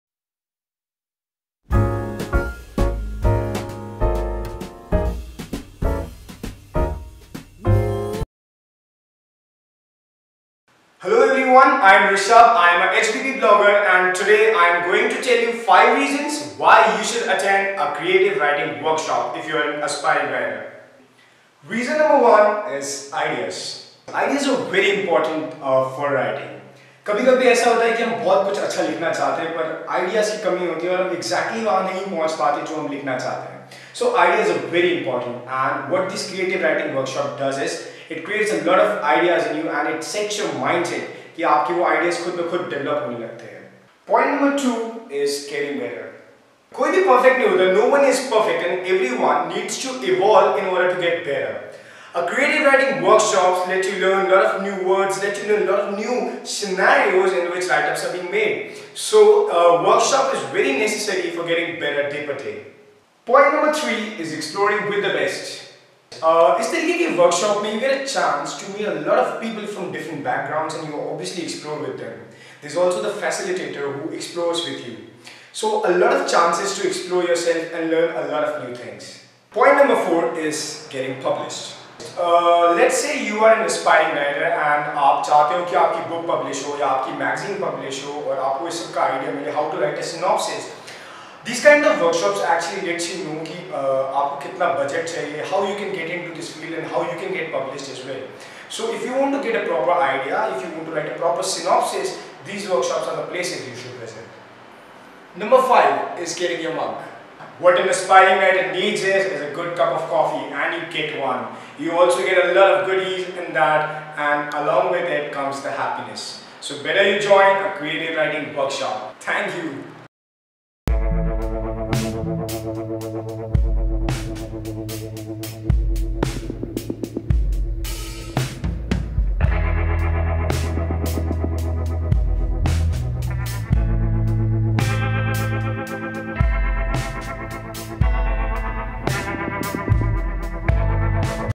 5 reasons why you should attend a creative writing workshop
If you are someone who has a penchant for writing and want to grow in the field , here are some reasons why you should attend such a workshop.

Video By HBB Blogger Rishabh Yadav